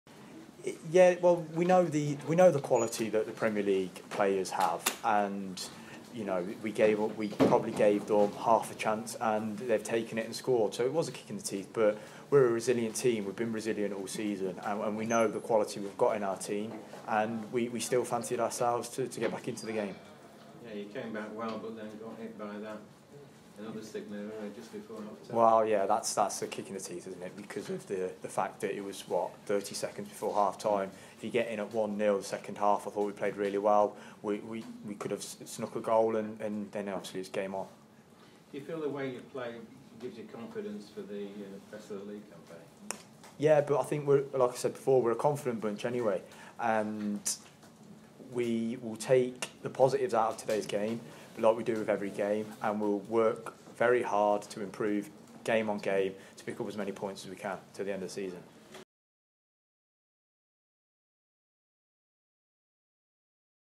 Matty Blair proud after display against Crystal Palace | iFollow Rovers
Matty Blair says Rovers can take real heart from their performance against Crystal Palace in the Emirates FA Cup.

First-half goals from Jeffrey Schlupp and Max Meyer saw Rovers bow out of the competition at the fifth round stage, but Blair believes they will learn a lot from it.